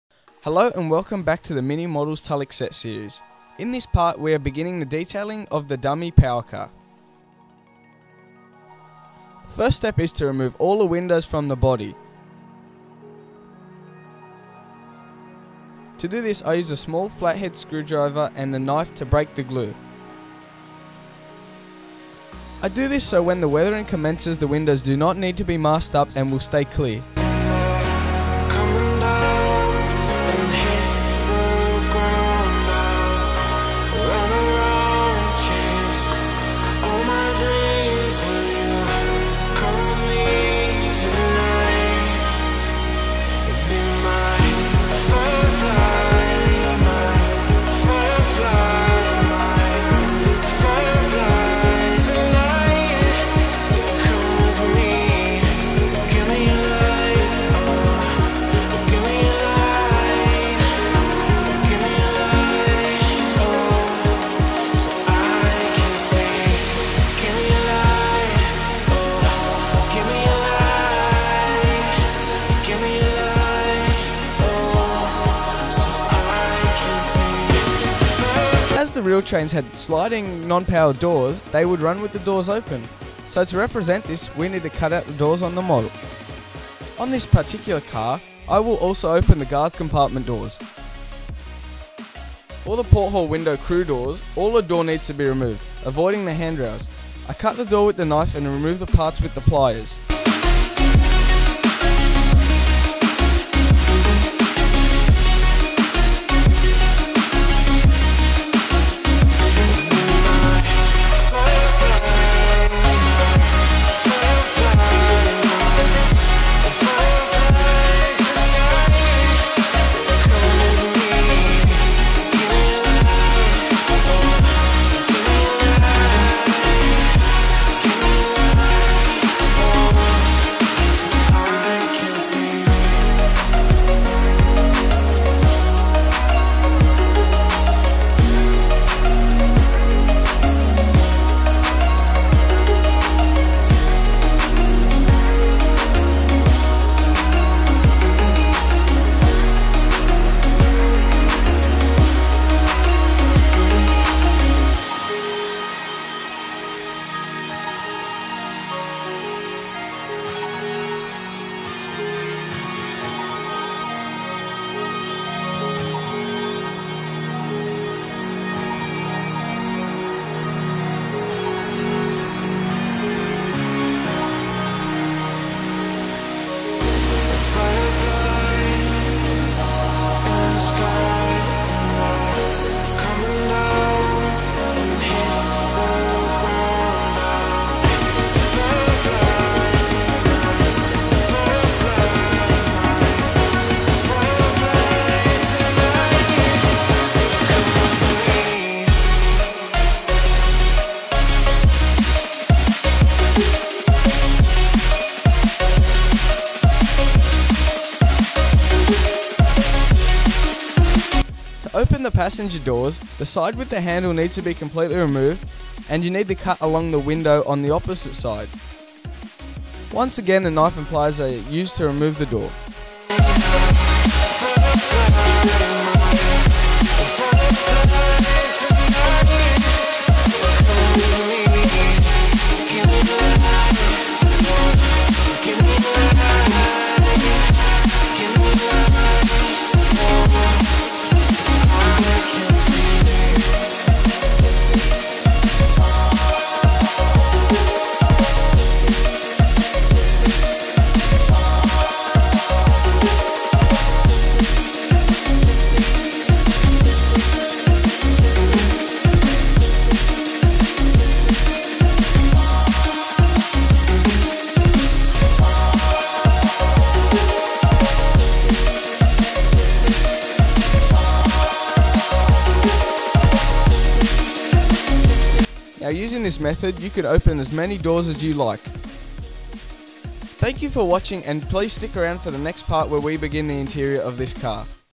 Minimodels Tulloch Set Part 2 - Doors
A different video for a change!

Welcome to the Tulloch set series!
Where I reveal some small and helpful secrets that I use to really complete these models. Ive been at it for about 8 years!
Model: 505 - Tulloch Indian Red

Brand: Minimodels